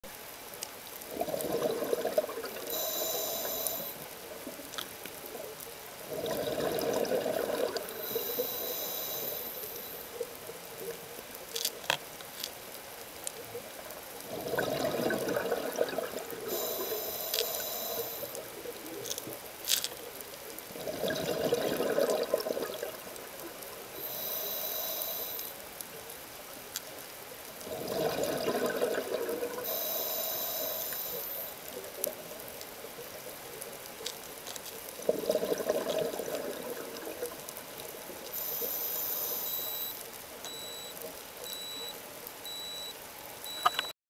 Bumphead Parrot Fish School in Indonesia
Bolbometopon muricatum Spotted while diving the Gili Islands in Indonesia